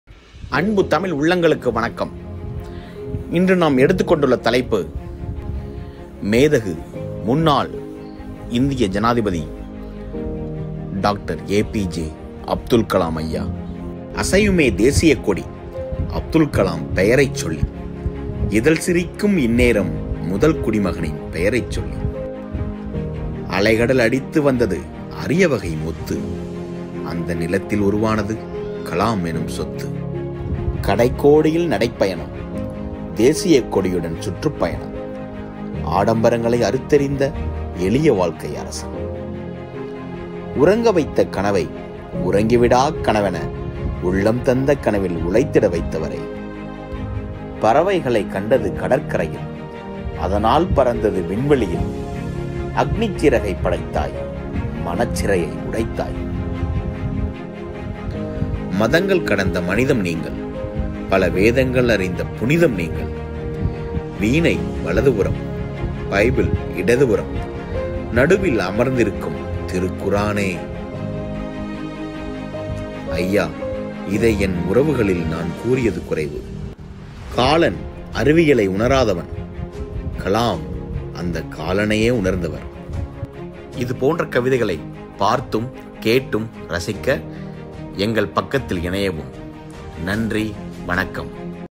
A. P. J. ABDUL KALAM THE UNTOLD POETRY - ஏ. பி. ஜே. அப்துல் கலாம் | VARIGAL - WILLIAM | Paa-பா
Avul Pakir Jainulabdeen Abdul Kalam 15 October 1931 – 27 July 2015  was an Indian aerospace scientist who served as the 11th President of India from 2002 to 2007. He was born and raised in Rameswaram, Tamil Nadu and studied physics and aerospace engineering.